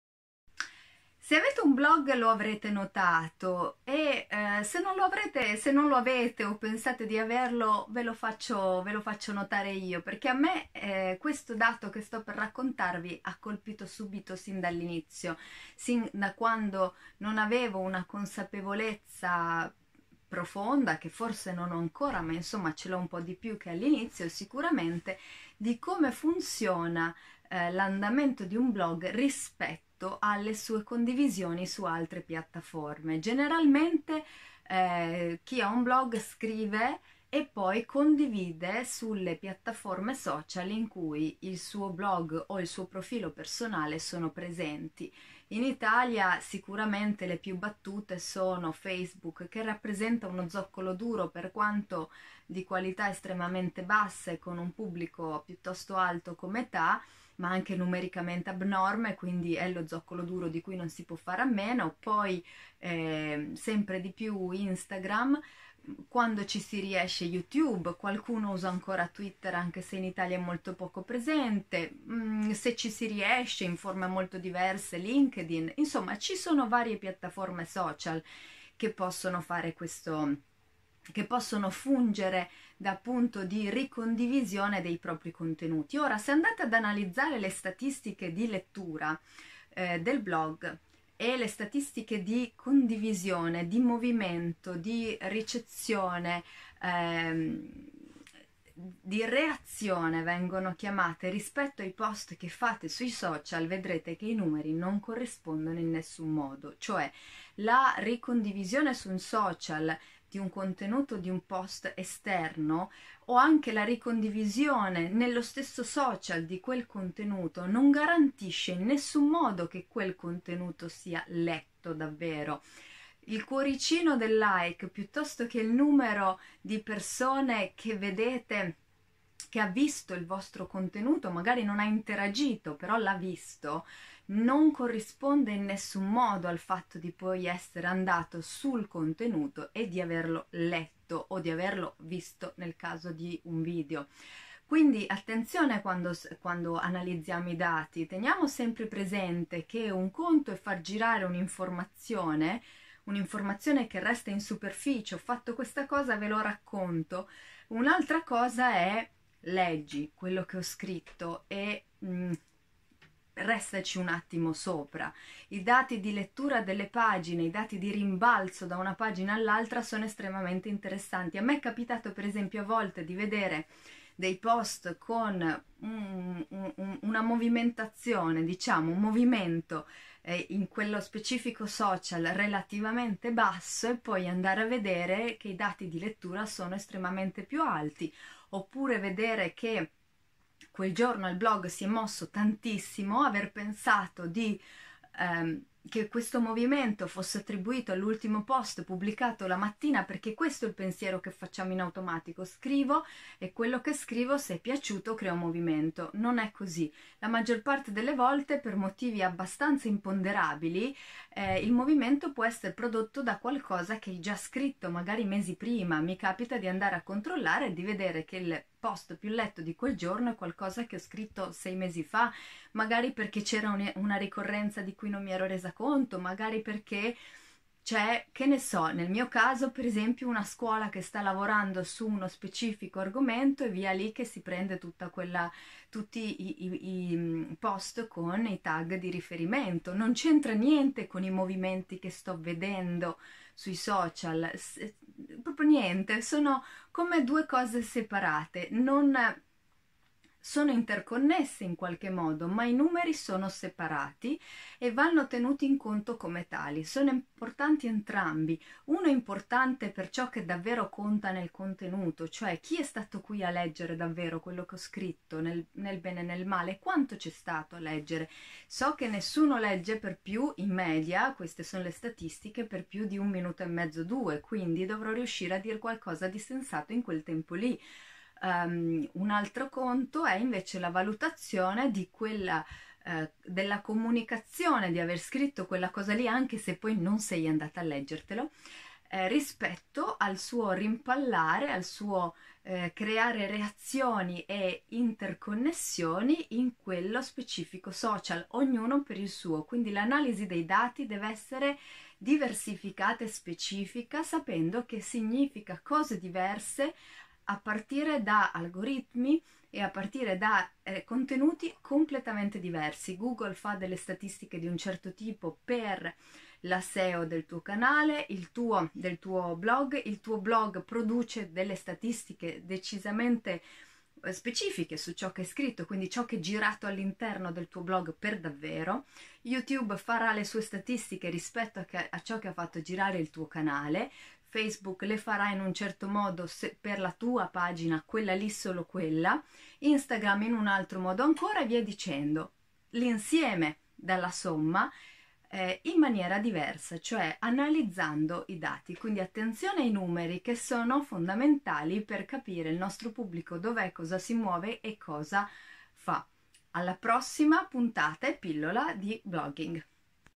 Dal blog alla condivisione sui social
Capita di notare che i numeri tra le statistiche di lettura e permanenza e rimbalzo sul blog siano molto diverse, sia in meglio che in peggio, rispetto a quelle della singola piattaforma social su cui il contenuto del blog è stato condiviso. Perché succede questo?

Vieni a scoprire i corsi on line dedicati al mondo della letteratura per bambini e ragazzi sul mio portale di e-learning